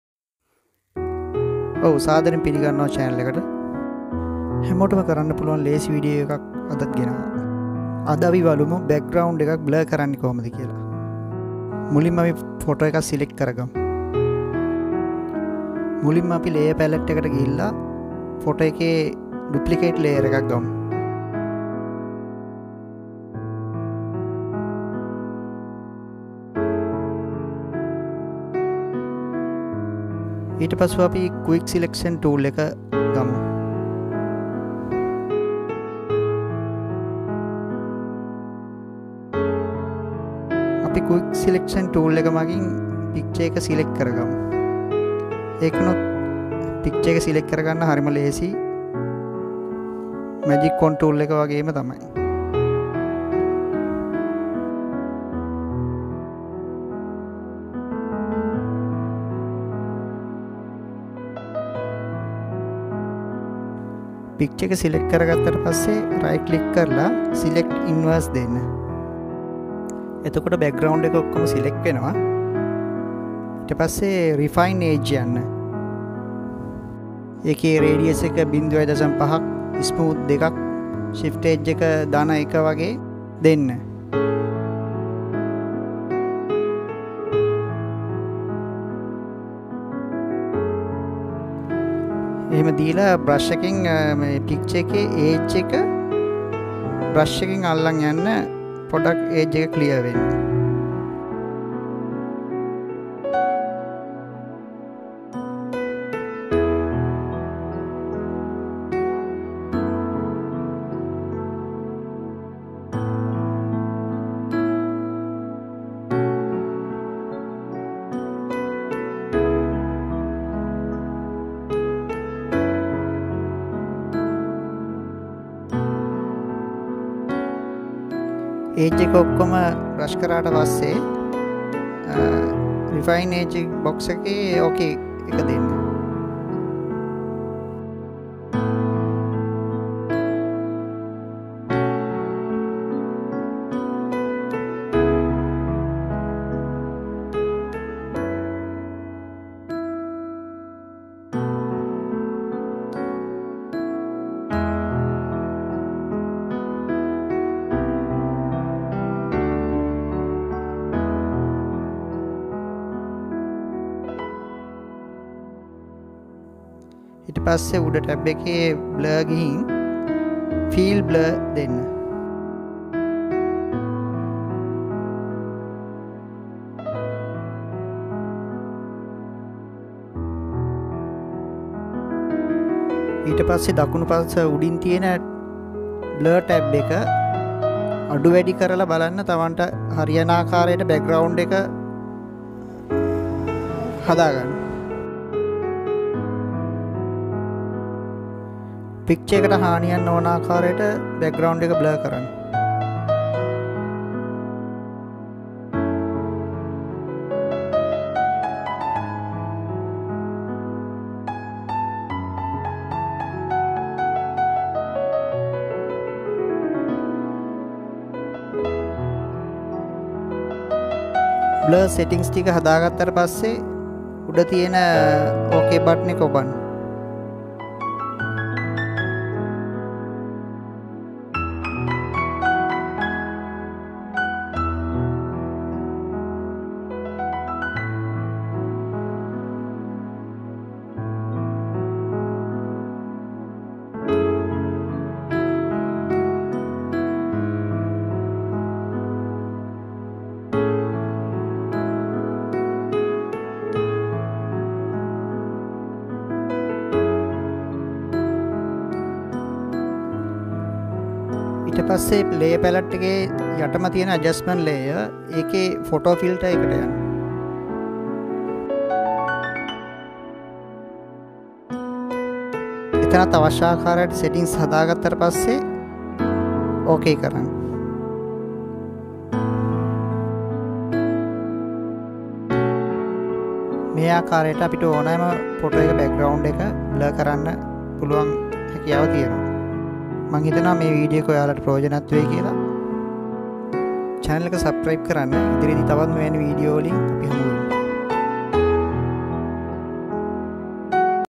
How to Blur Photo Background in Photoshop , Blur your Photo Background Easily
In this photo editing tutorial, learn how to add a smooth blur to your photo backgrounds easily in Photoshop. It's Very Easy to beginner

Software Used:
Photoshop  cs6,